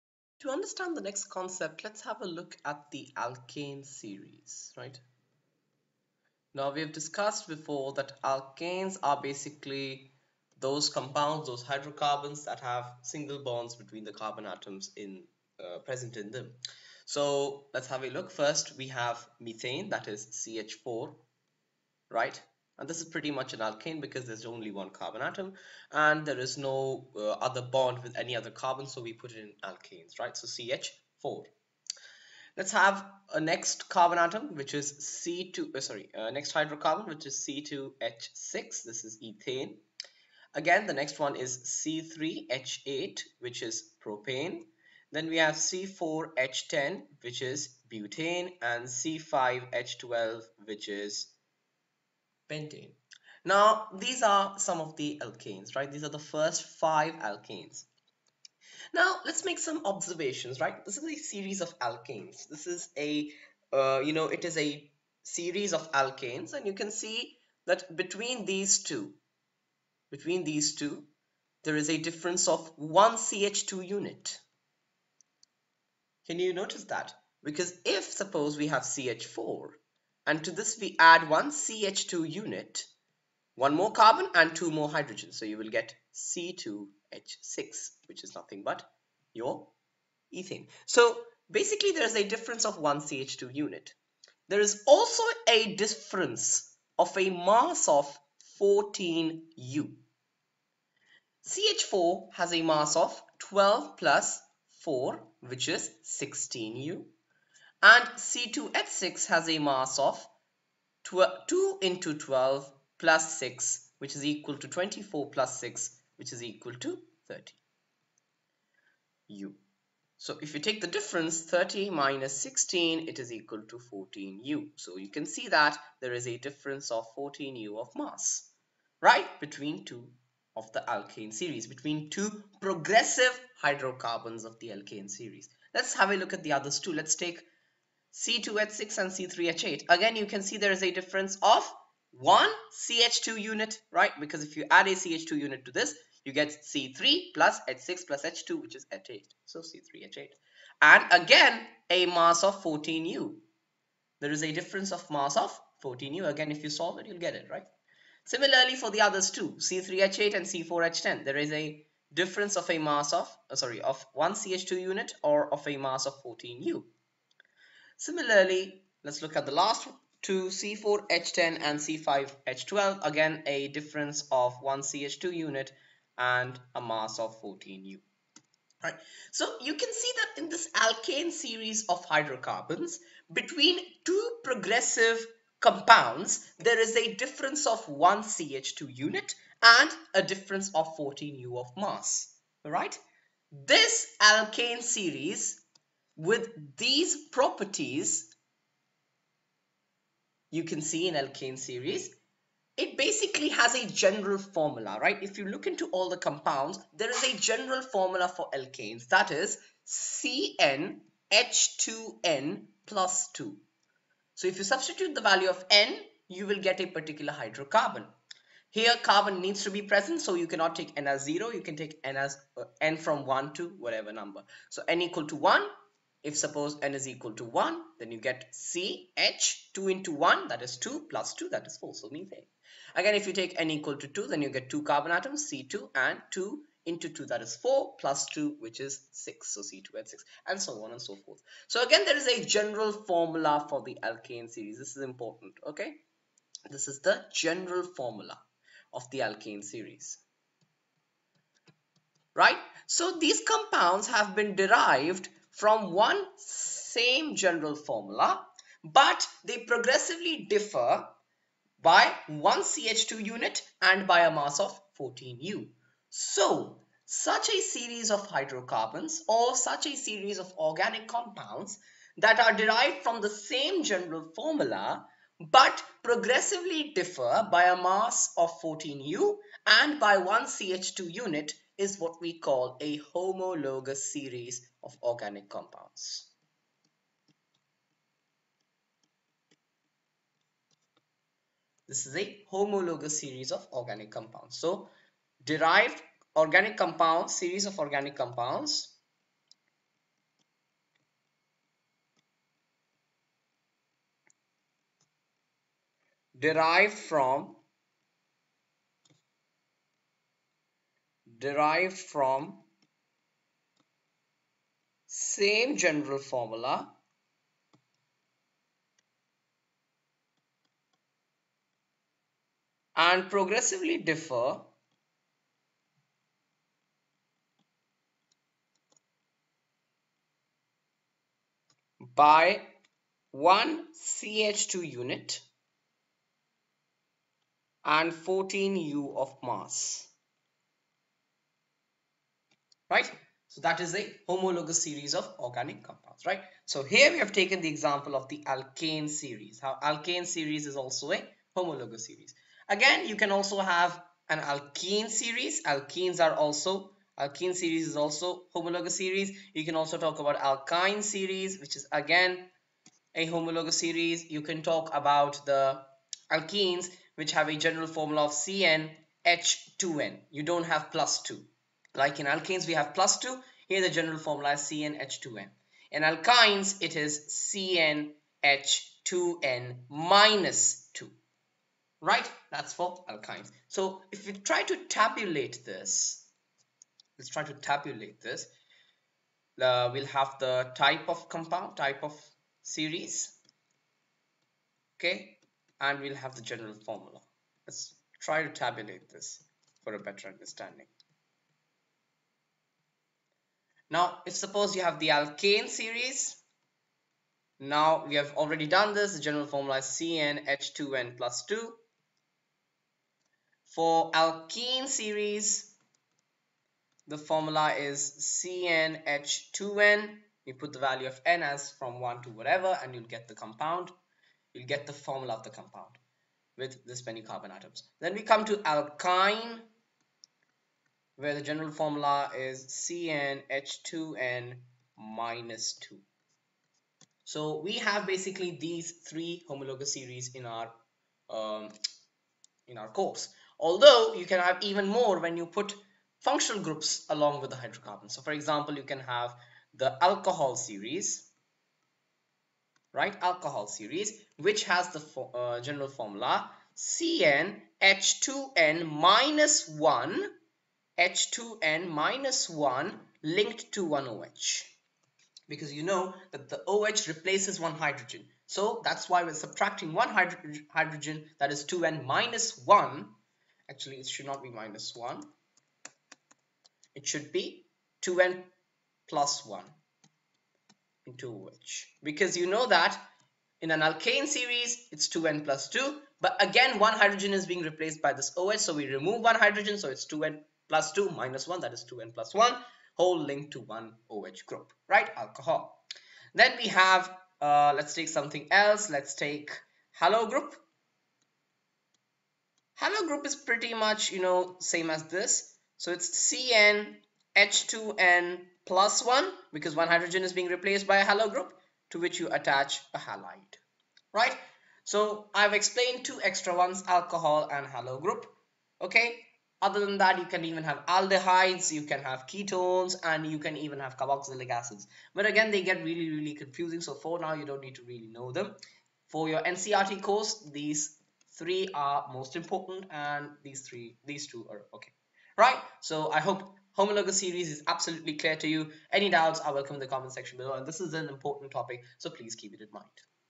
Class 10 Carbon and its Compounds-Homologous series
This video lesson talks about the concept of Homologous Series of Organic Compounds, and its characteristics. A Homologous Series of Compounds is the series of organic compounds that have been derived from the same general formula, but progressively differ by one -CH2 unit and 14u of mass. The general formula of Alkanes, Alkenes and Alkynes has been discussed in this lesson.